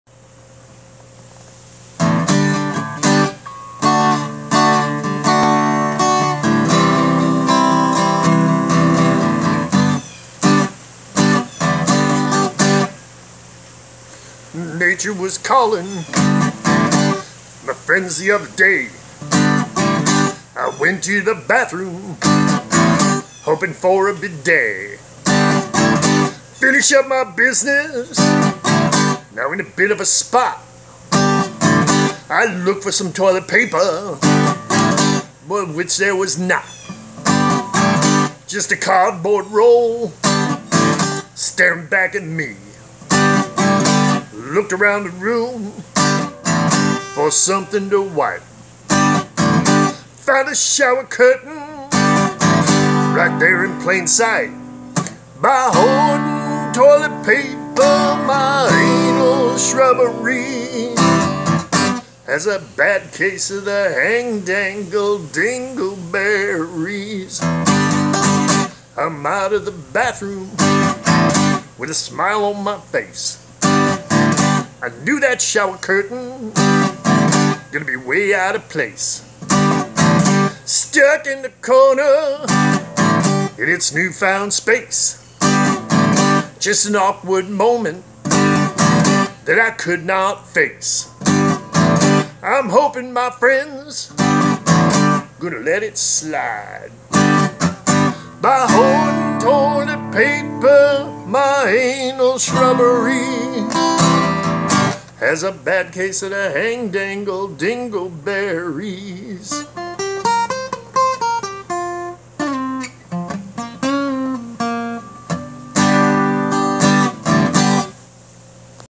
Toilet paper hoarding hang dangle dingleberries
Blues riff about missing toilet paper